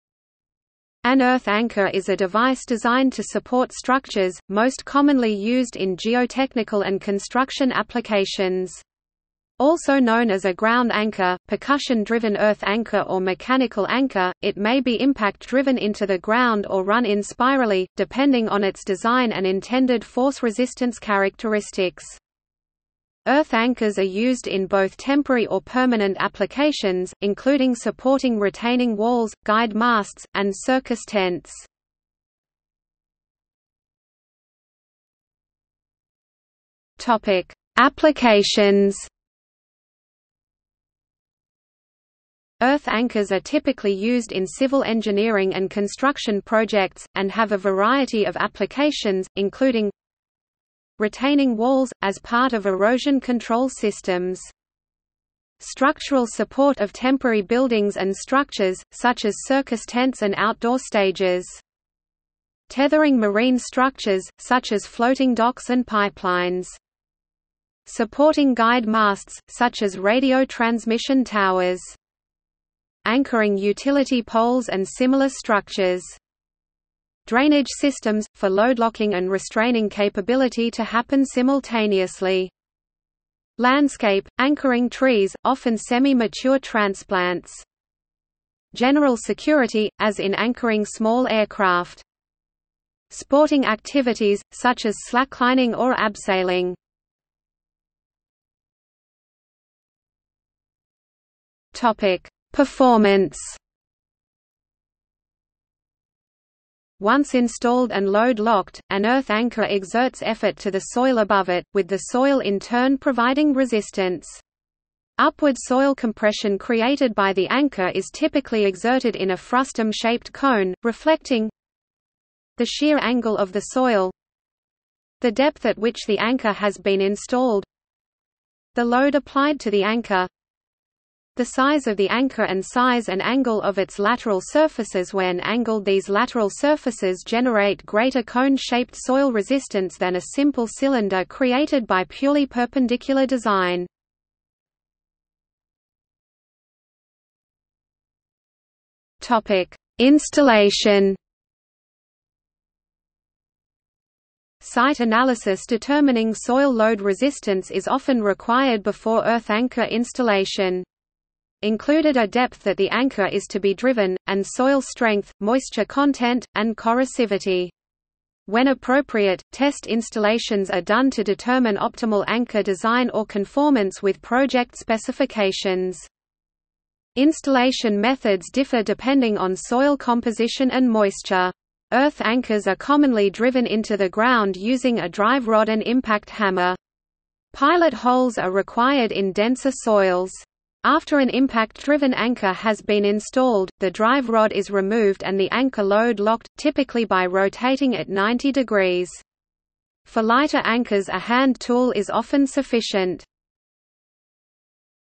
Earth anchor | Wikipedia audio article
00:00:33 1 Applications
00:01:38 2 Performance
00:02:25 3 Installation

- Socrates

SUMMARY
An earth anchor is a device designed to support structures, most commonly used in geotechnical and construction applications. Also known as a ground anchor, percussion driven earth anchor or mechanical anchor, it may be impact driven into the ground or run in spirally, depending on its design and intended force-resistance characteristics.
Earth anchors are used in both temporary or permanent applications, including supporting retaining walls, guyed masts, and circus tents.